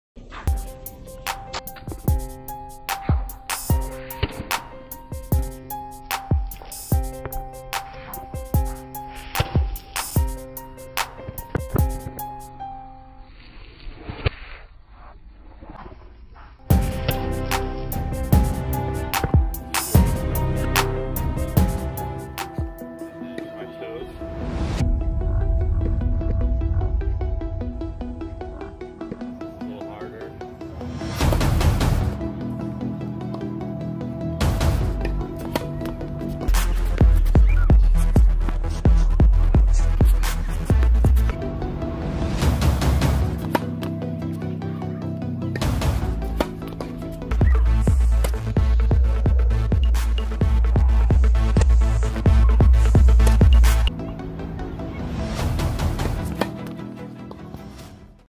Tennis Serves and Shadow Swings in Las Vegas
Tennis Coach Andre Schlessinger serving and forehand-ing tennis balls on the tennis court in Las Vegas.